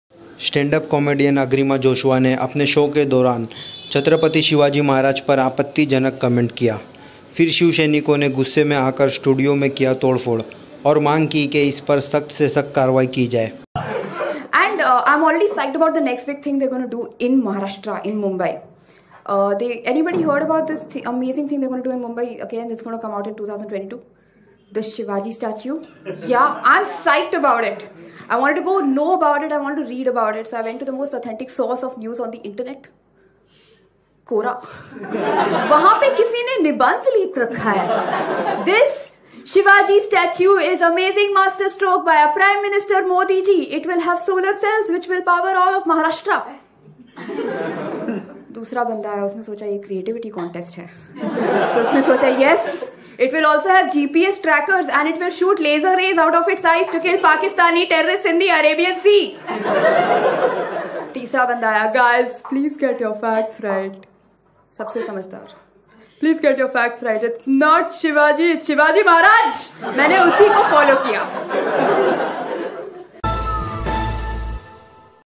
Comedian ne Chhatrapati Shivaji Maharaj per Apatti Janak Coment kiya.
*SHIVSAINIKO NE KI STUDIO ME TOD FOD, COMEDIAN AGRIMA JOSHUA NE SHIVAJI MAHARAJ PER KIYA THA APATTI JANAK COMMENT.*

*KUCHH SAMAY PEHLE STANDUP COMEDIAN AGRIMA JOSHUA NE CHHATRAPATI SHIVAJI MAHARAJ KO LEKAR SHOW KE DAURAN APATTI JANAK COMMENT KIYA THA. JIS KA VIDEO AB SOCIAL MEDIA PAR KAFI VIRAL HO RAHA HAI.*

*VIDEO VIRAL HONE KE BAAD COMEDIAN KI IS HARKAT PER GUSSA SHIVSAINIKO NE STUDIO ME JAMKAR TOD POD MACHA KI.*

*SHIVSENA MLA PRATAB SARNAIK NE MAHA H.M ANIL DESHMUKH SE US COMEDIAN KE KHILAF SAKHT KARWAHI KARNE KI APPEAL KI HAI.*

*JIS KE BAAD MAHA H.M ANIL DESHMUKH NE CP MUMBAI AUR IG CYBER K COMEDIAN KE KHILAF LEGAL ACTION LENE KE INSTRUCTIONS DIYE HAI.*

*BADHTE BAWAL KO DEKHTE HUE COMEDIAN AGRIMA JOSHUA NE APNI IS HARKAT PER MAAFI BHI MANGI HAI.*